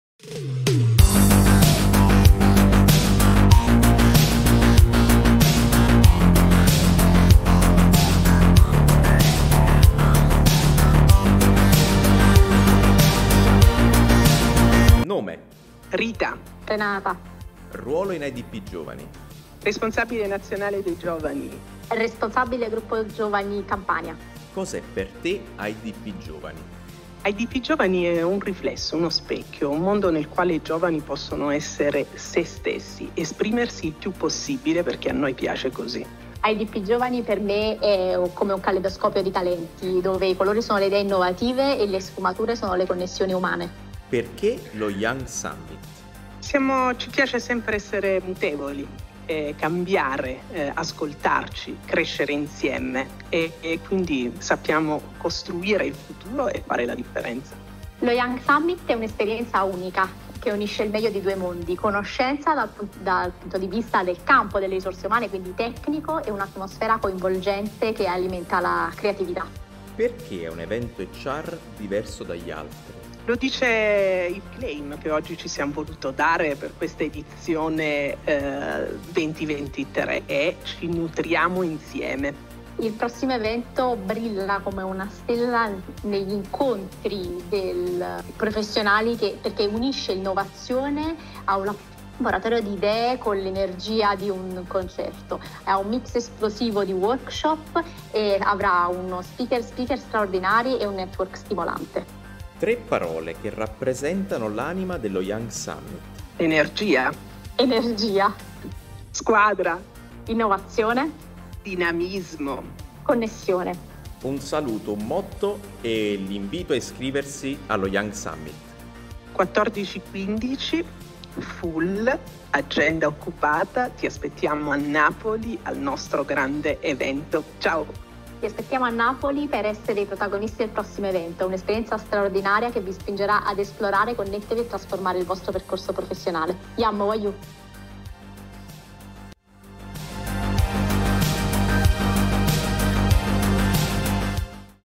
AIDP Young Summit 2023 NutriAMOci Intervista a Renata Rallo e Rita Melcarne
Intervista a Renata Rallo e Rita Melcarne sulla quinta edizione dell'AIDP Young Summit.
Vi aspettiamo a Napoli 14 e 15 ottobre 2023!